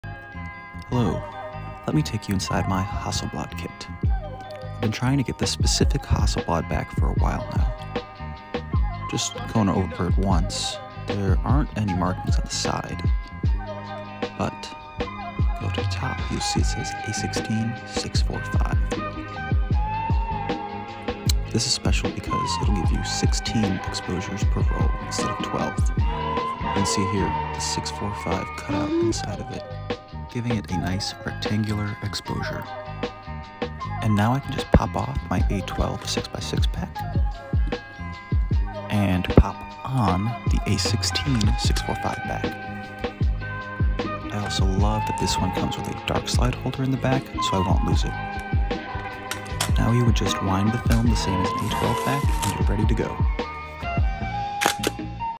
Turning my Hasselblad 500 into a 645 camera with the A16 film back
The newest addition to my Hasselblad 500 camera kit is a back that gives me 4 extra exposures per roll, helping me save money shooting film. The A16 back crops each shot down to 645 to give you more exposures with the same body and lenses I already own, instead of needing to buy a new 645 camera.